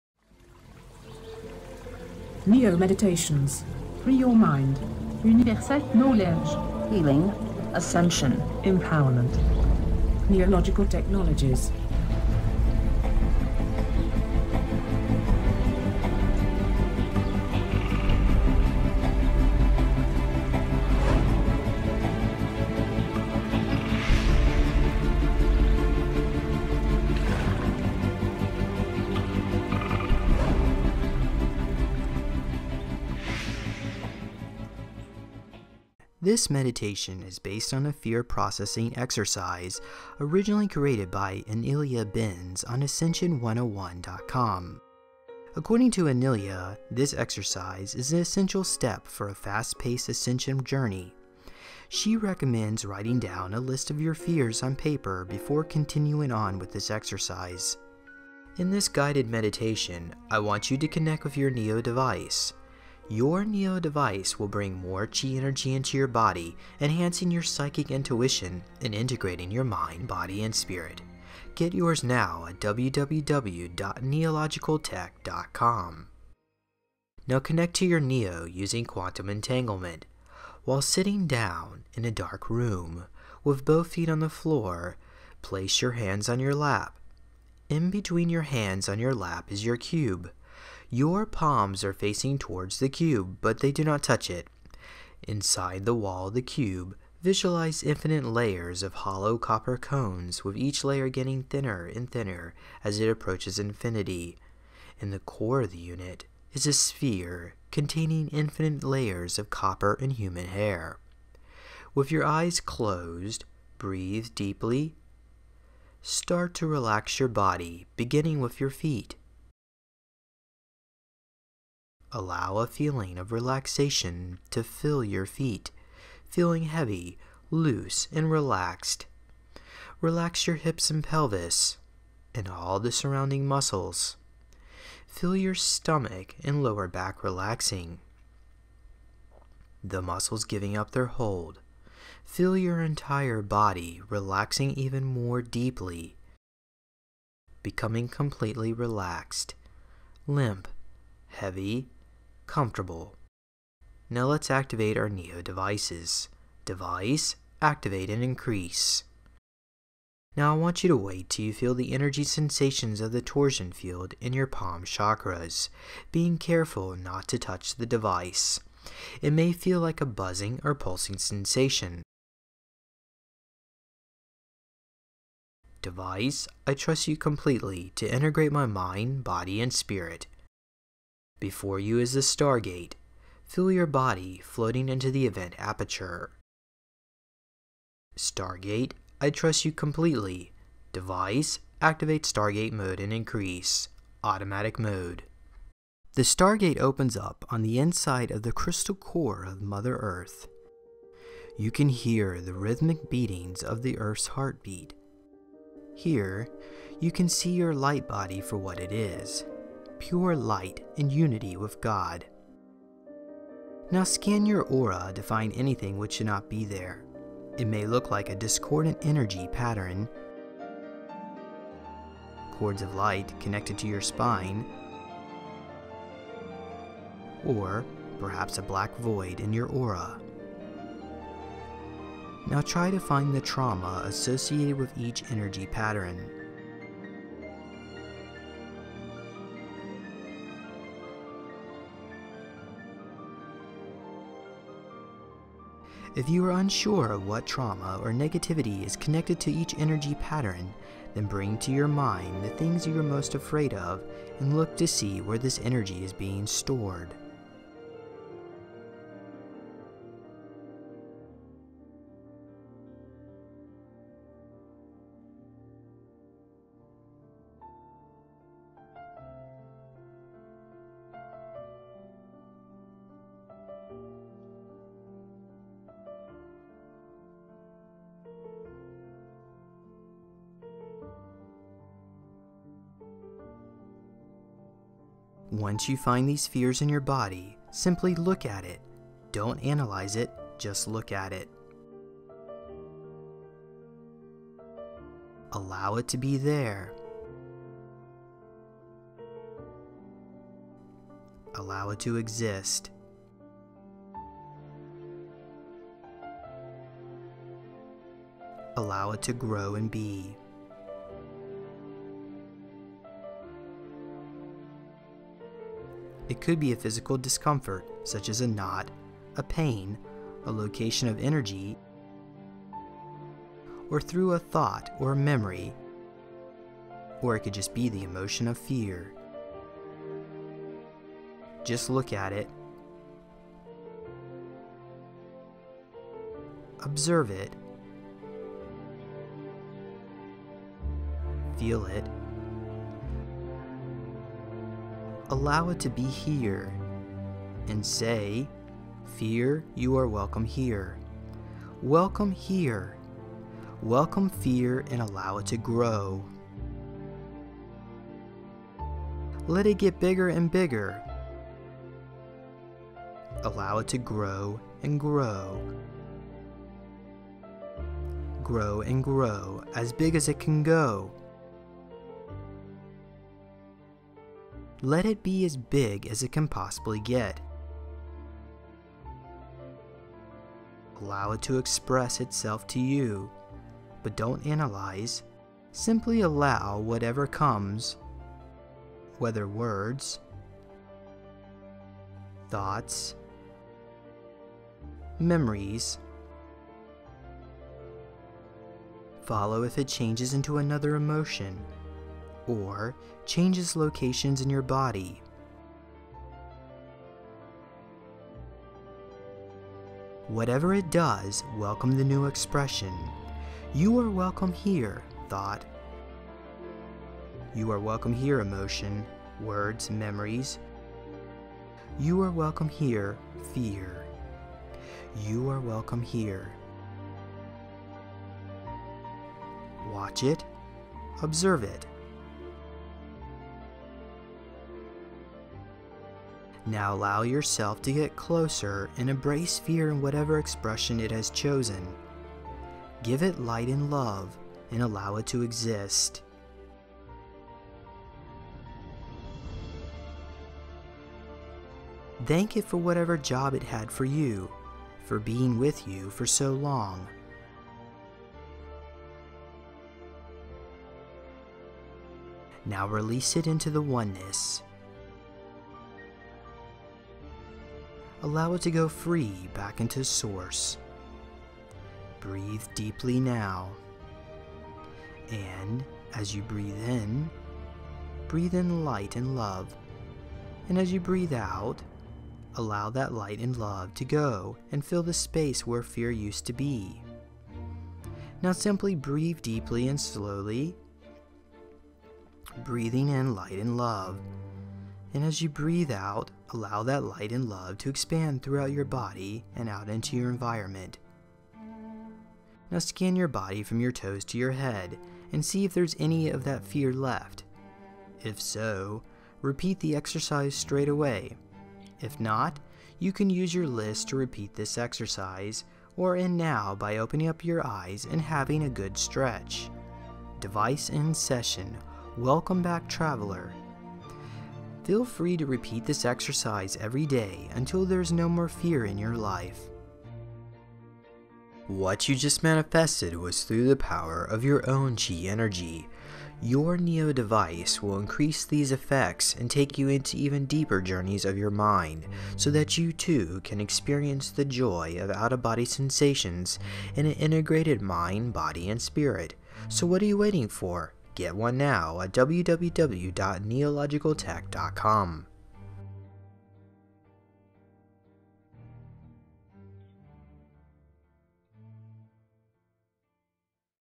Conquering Your Fear Neo Meditation
This meditation is based on a fear processing exercise originally created by Inelia Benz on ascension101.com . According to Inelia this exercise is an essential step for a fast pace ascension journey. She recommends writing down a list of your fears on paper before continuing on in this exercise.

Original Air Date: Friday, June 7, 2012
Hosted by: James Rink

Free yourself from fiat money with your own gold back debit card